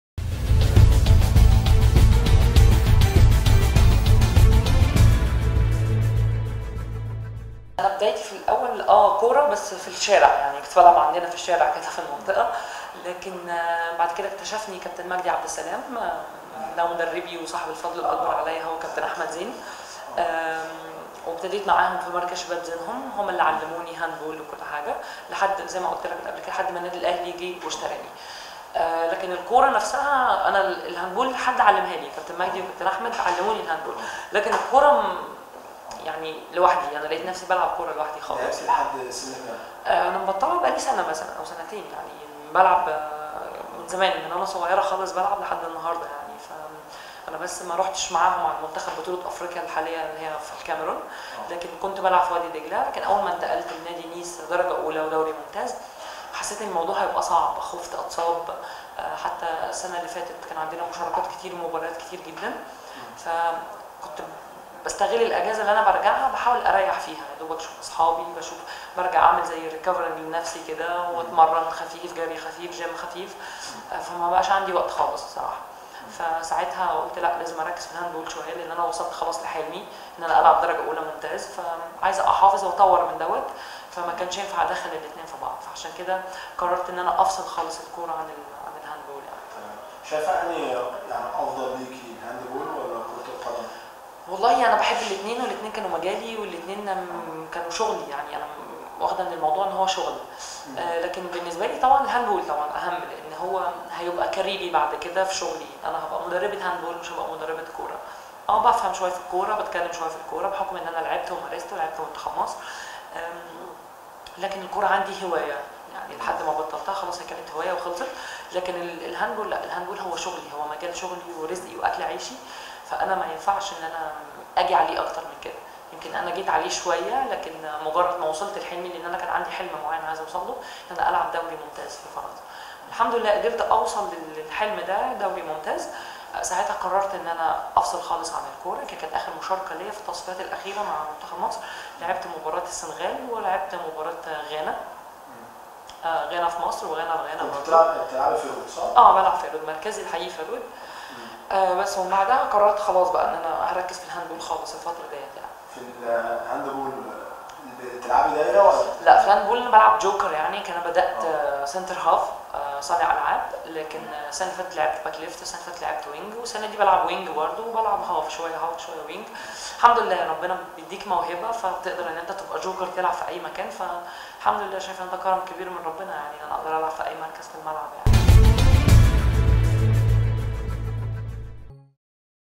مصر العربية | مروة عيد: بدأت رياضة بلعب الكورة في الشارع
كشفت مروة عيد ﻻعبة كرة اليد المحترفة في صفوف نيس الفرنسي، أنها استهلت ممارسة الرياضة بلعب كرة القدم في الشارع منذ الصغر واستمرت في ممارسة كرة القدم مع تعلمها كرة اليد، حتى العام الماضي.